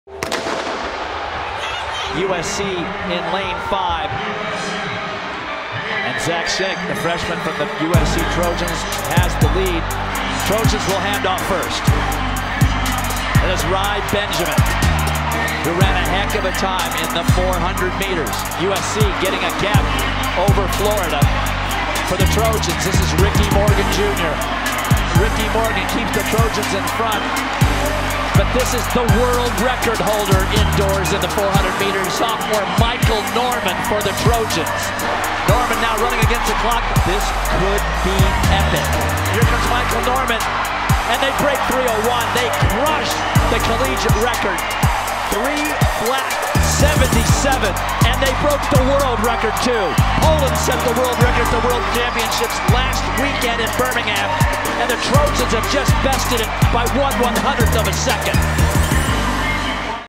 Highlight: USC's 4x400m relay team sets world record at NCAA Indoor Track & Field Championships
USC's 4x400m relay team of Zach Shinnick, Michael Norman, Rai Benjamin and Ricky Morgan Jr. set a world record Saturday with a time of 3:00.47 at the NCAA Indoor Track & Field Championships in College State, Texas.